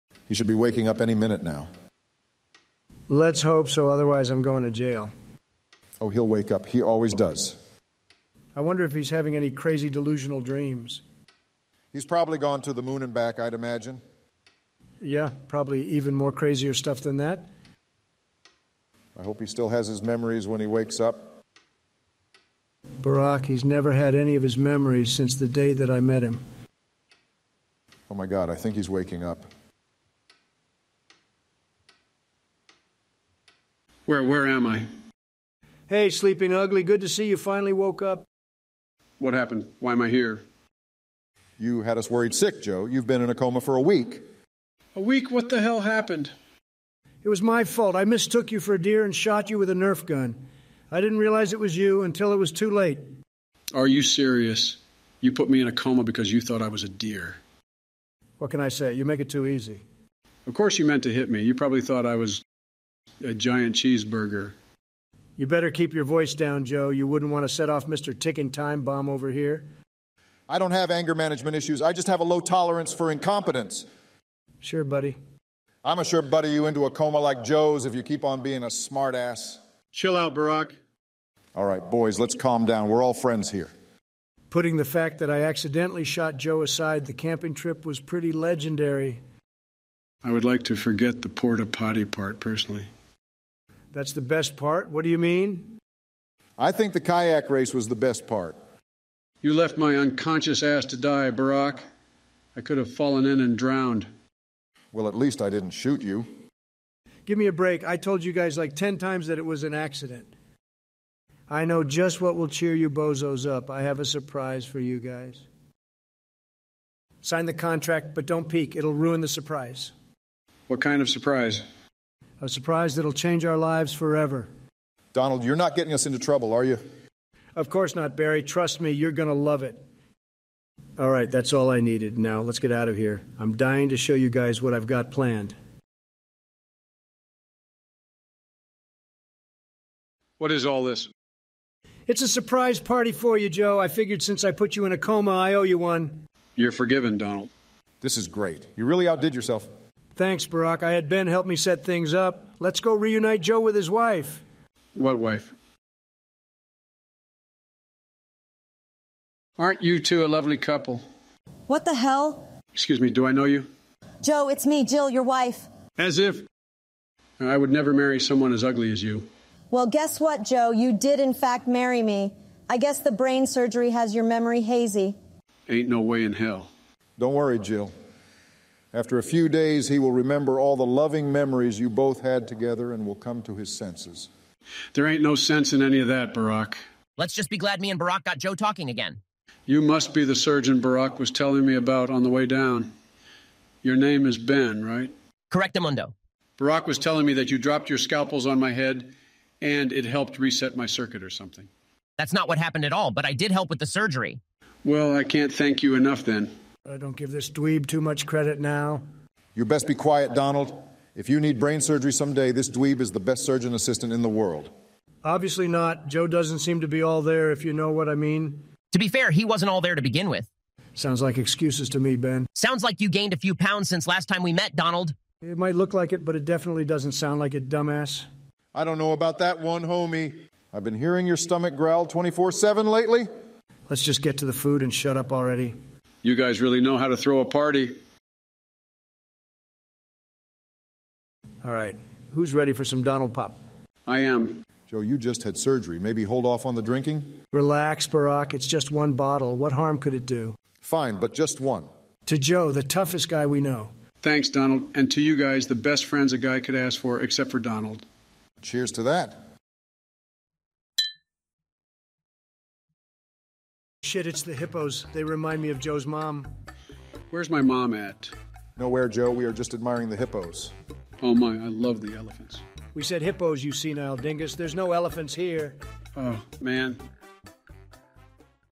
Sleepy Joe Finally Wakes Up...(AI Presidents Story) Season 2 Finale!
After being shot with a nerf gun, our beloved Joe has been in a coma in the hospital. After being in the coma for a week, Joe finally wakes up!

All voice lines used are AI generated. All characters are fictional. Neither the voice lines, characters, or anything else in my videos are meant to represent any real person or people.